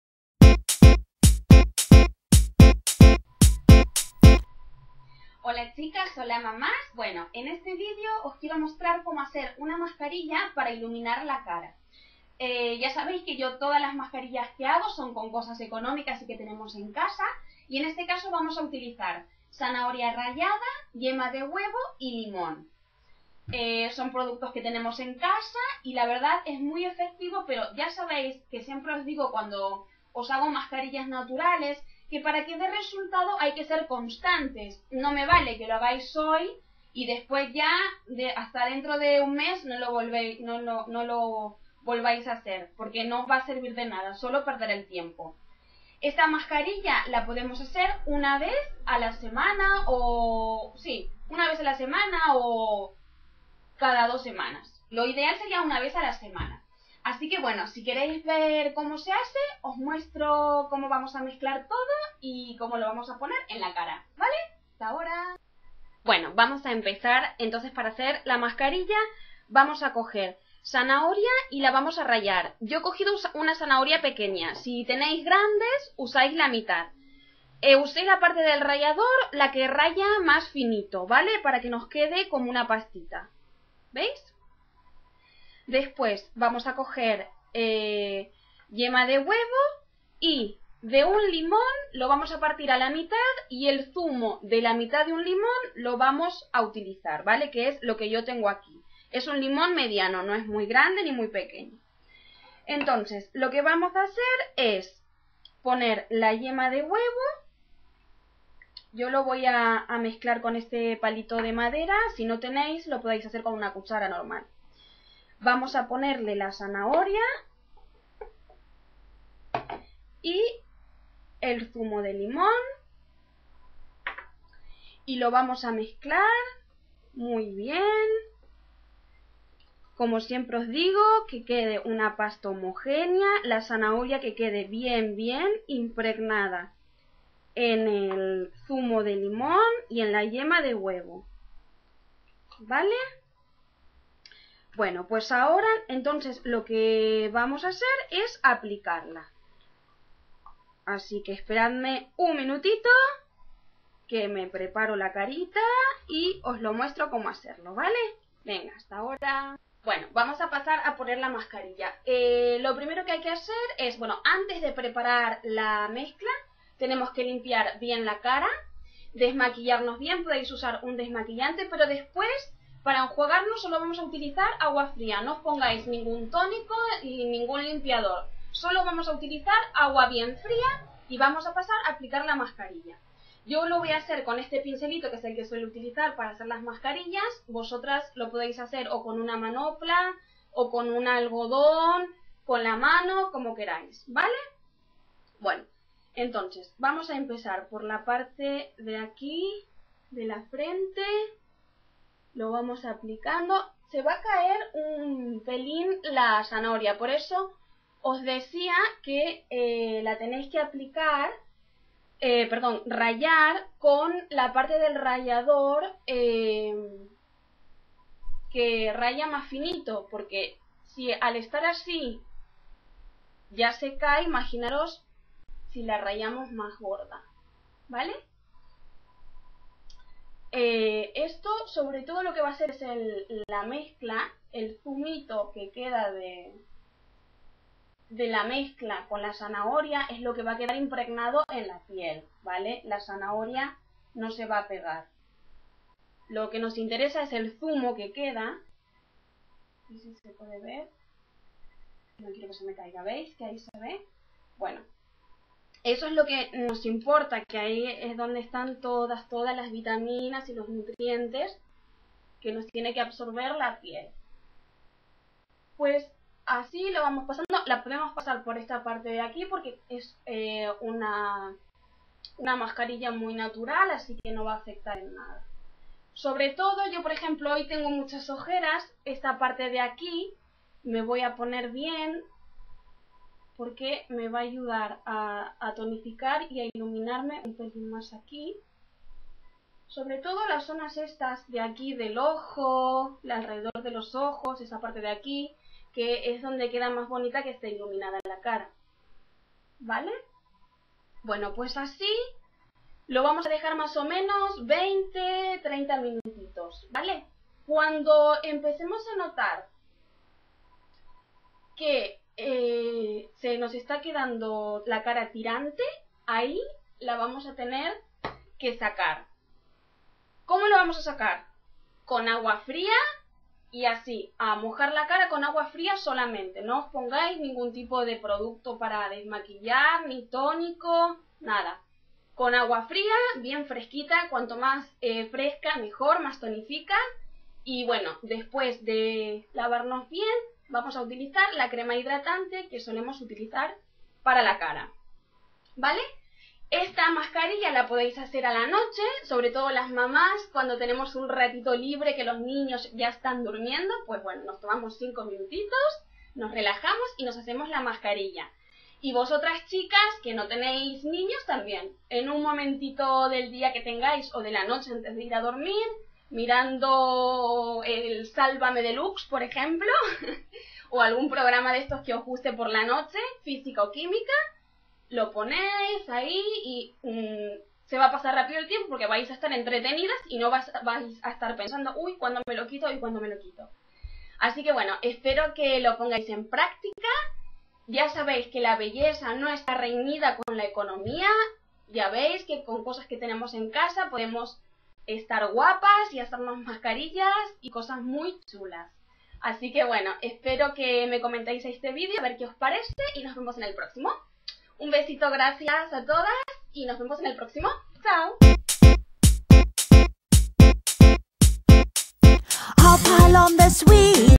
MASCARILLA NATURAL PARA ILUMINAR EL ROSTRO
OS ENSEÑARE A PREPARAR UNA MASCARILLA CASERA Y SUPER EFECTIVA PARA ILUMINAR LA CARA.
COMO SIEMPRE OS DIGO LA BELLEZA NO ESTA REÑIDA CON LA ECONOMIA! ASI QUE ADELANTE A PONEROS GUAPAS!
ME PODEIS SEGUIR TAMBIEN EN TWITTER